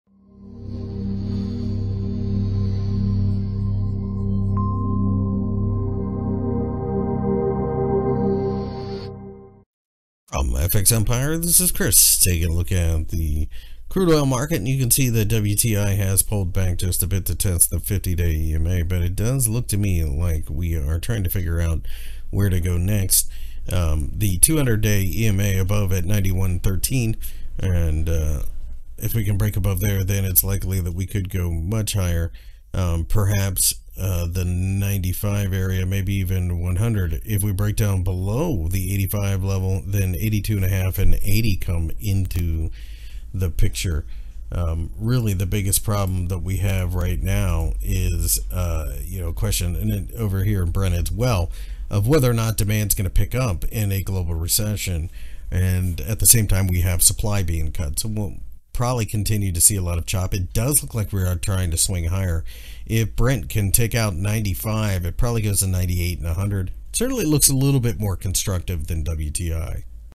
Oil Technical Analysis for November 04, 2022 by FXEmpire
Crude oil markets have pulled back just a bit during the trading session on Thursday as we continue to see a lot of choppy behavior in general.